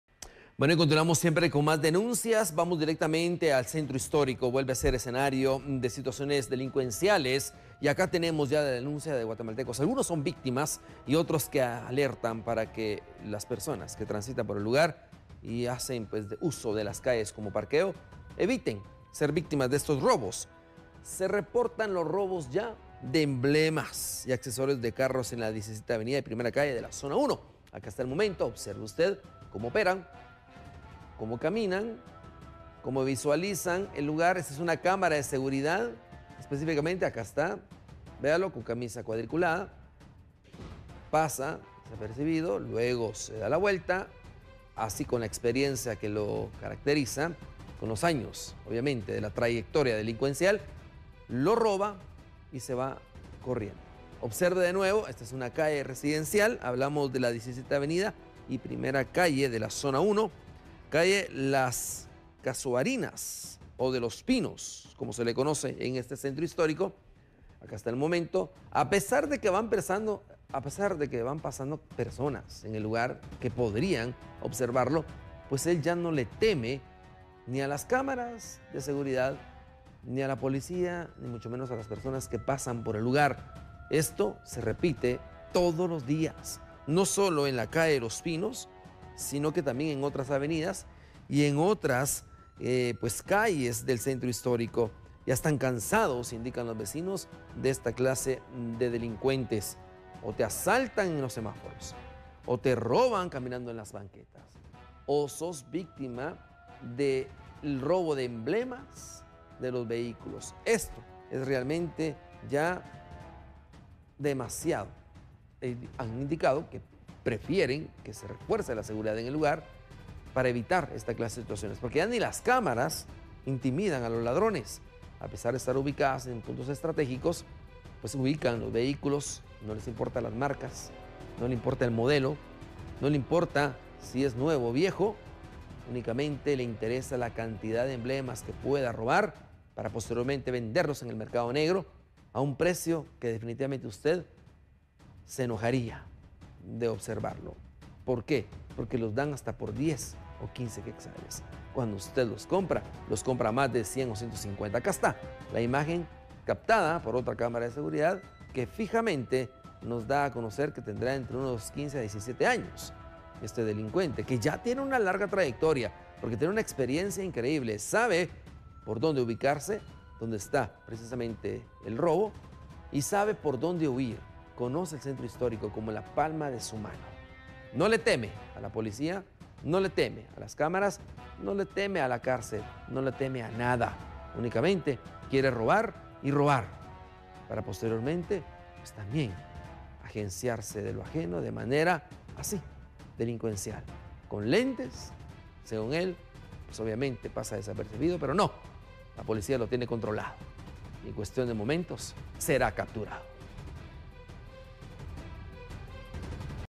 Denuncian el robo de emblemas y accesorios de un auto
En nuestro segmento 'Yo denuncio', le mostramos cómo una cámara de seguridad registra a un grupo de delincuentes que hurtan accesorios de carros en la 17° avenida y 1° calle, zona 1 del Centro Histórico de la capital.
Los vecinos indicaron que están hartos de la inseguridad en la zona.